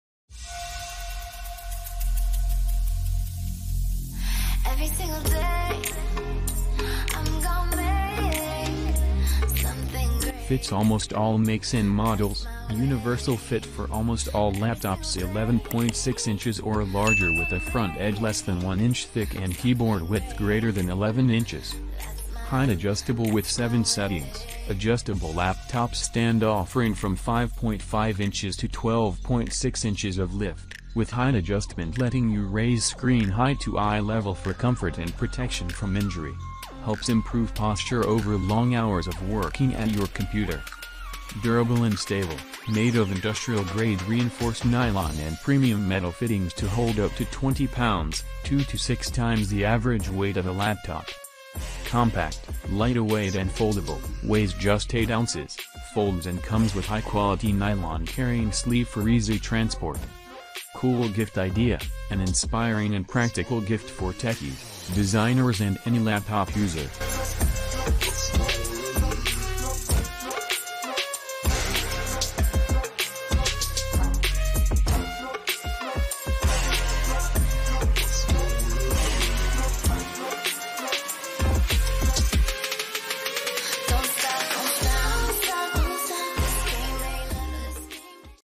Portable Laptop Stand - Adjustable Laptop Stand For Pc & Mac
Nexstand Laptop Stand

Description:
The Nexstand Laptop Stand is a portable and foldable MacBook stand and PC stand. Adjustable height settings and a universal fit make this ergonomic laptop stand the perfect stand for propping up almost all makes and models of laptops.

Get anywhere from 5.5 inches to 12.6 inches of life for your Apple or PC laptop. The stand features 8 height adjustments to let you raise the screen height to your eye level for maximum comfort. Better ergonomics with this laptop stand mean you can sit in front of your laptop for longer.

The Nexstand Laptop Stand has universal fit for almost all laptops 11.6 inches and larger with a front edge less than 0.75 inches thick and a keyboard width greater than 11 inches. The stand is ideal for ASUS, HP, Samsung, Dell, Matebook, Surface, Lenovo, Acer, ThinkPad, and Apple MacBook laptops.